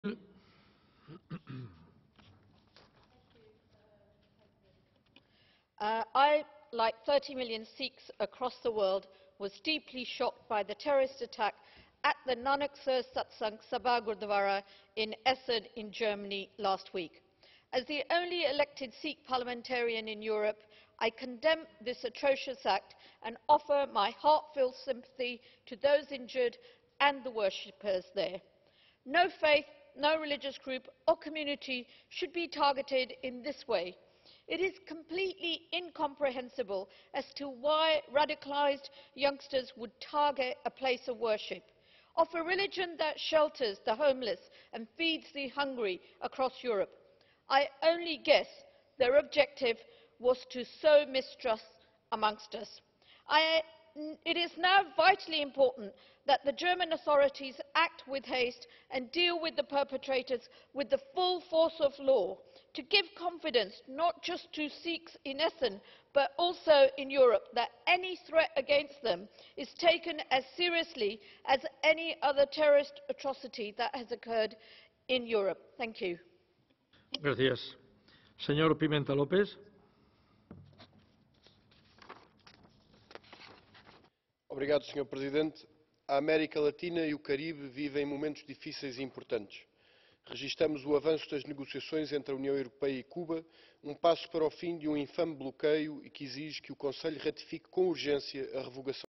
Neena Gill 27.04 Speech on Terrorist Attack on Gurdwara in Essen, Germany
One Minute Speech on attack on German Gurdwara in Essen